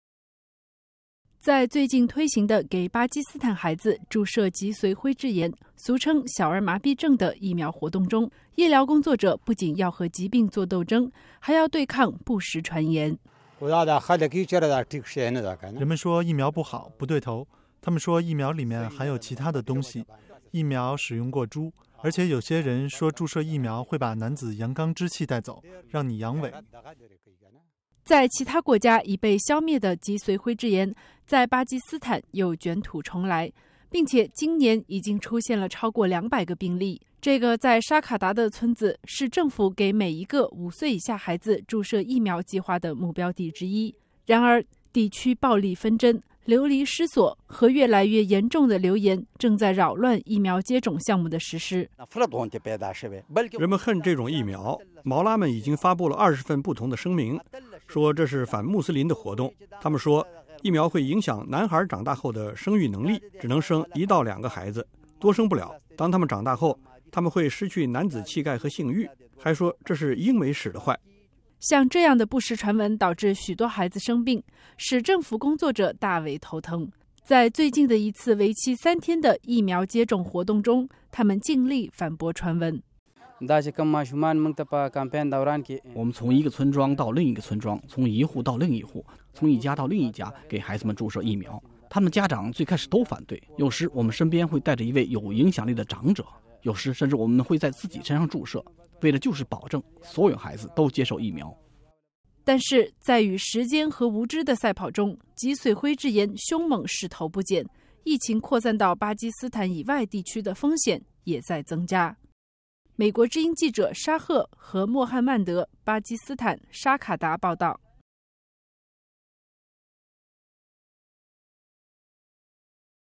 谣言导致巴基斯坦脊髓灰质炎疫情恶化
巴基斯坦在对抗脊髓灰质炎的战斗中不断受挫， 迎来了近十多年来最严重的一次疫情爆发。在美国中央情报局利用一支疫苗组来监测本•拉登的行踪之后，人们开始怀疑医疗工作者是间谍,这种疑心让治疗脊髓灰质炎的进程更加受挫。